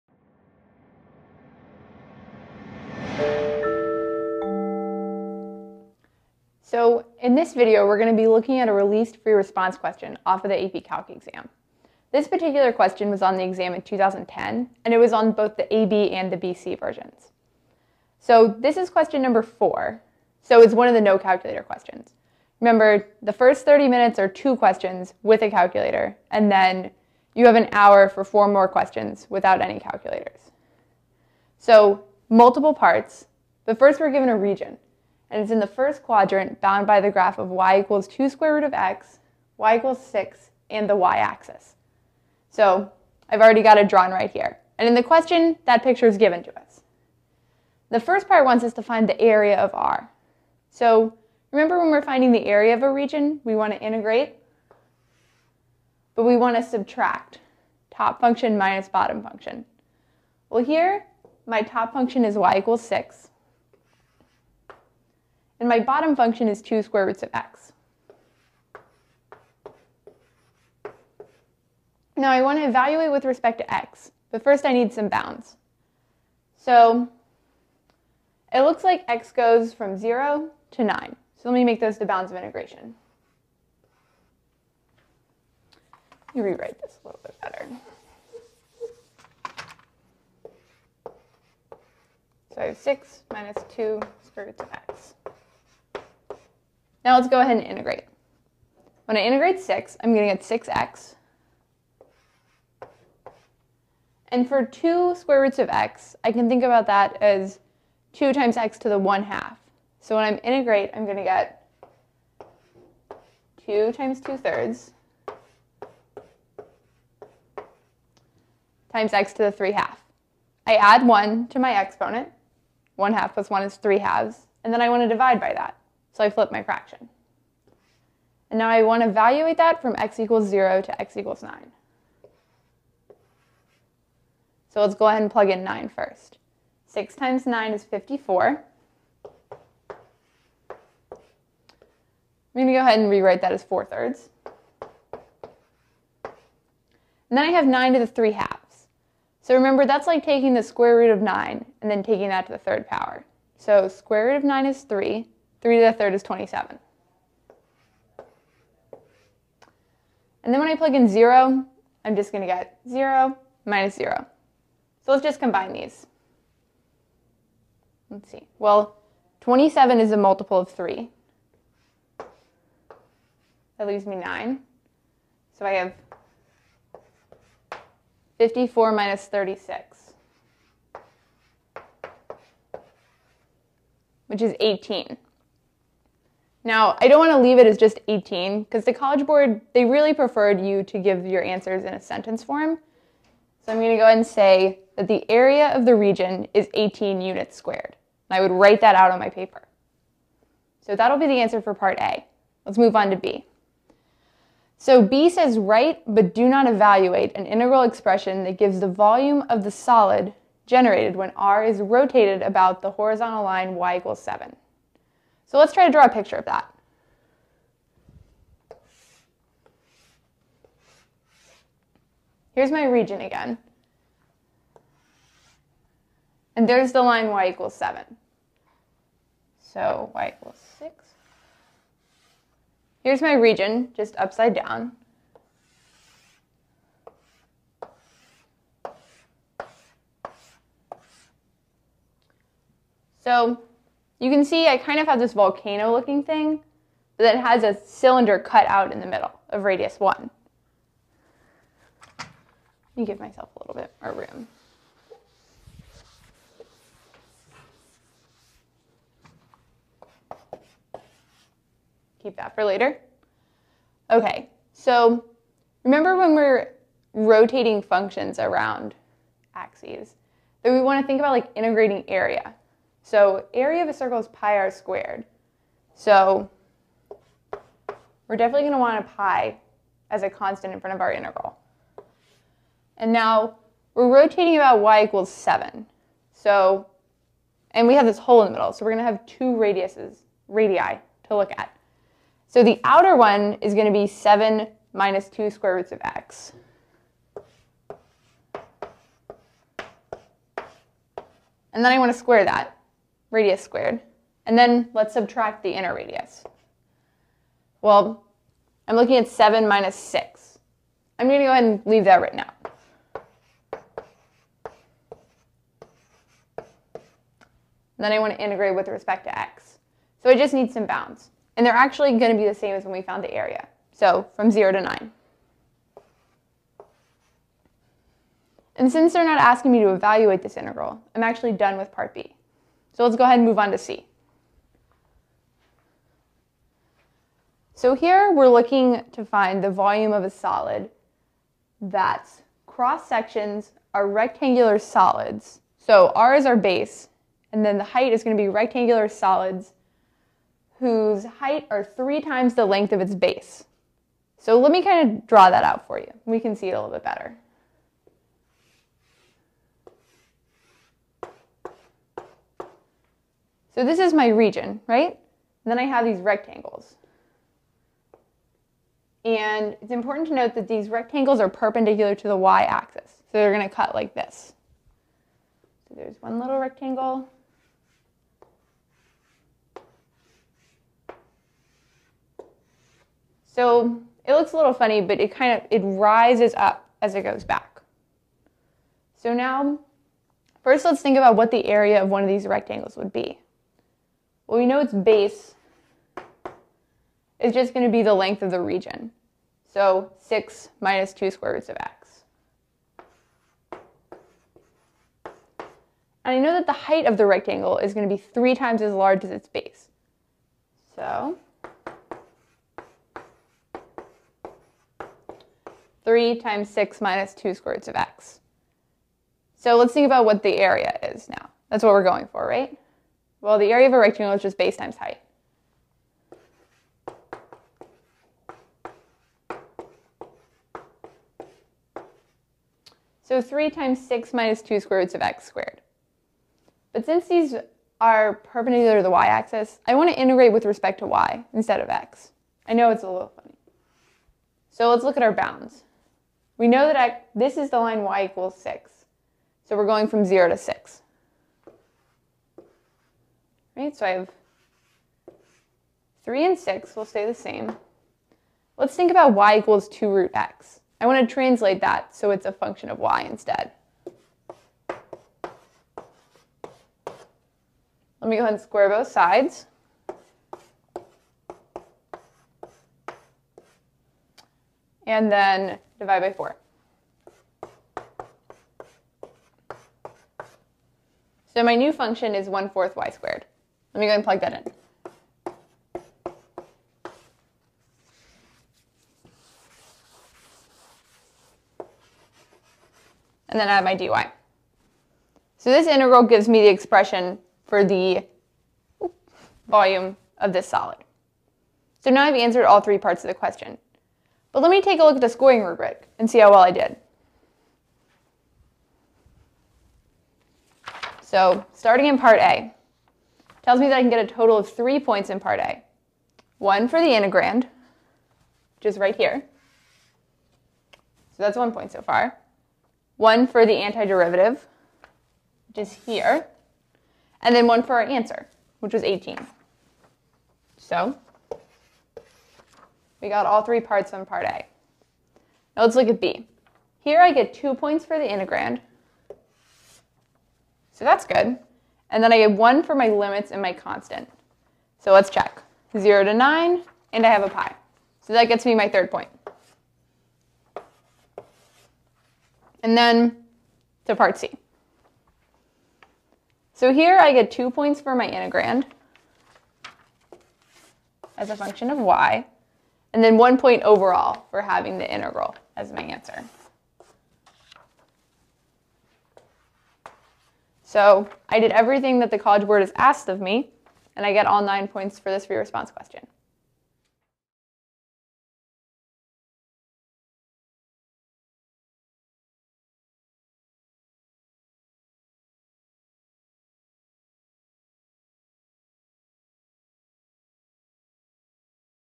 AP Calculus Free Response Question: Area and Volume
This video helps students work through a free response question from the AP Calculus exam that deals with area and volume. The free-response section of the AP Calculus exam tests your ability to solve problems using an extended chain of reasoning and contains six questions: two that allow the use of a calculator and four that do not. The problem in this video does not permit the use of a calculator and was on both the AB and BC versions of the exam. For more AP Calculus videos visit centerofmath.org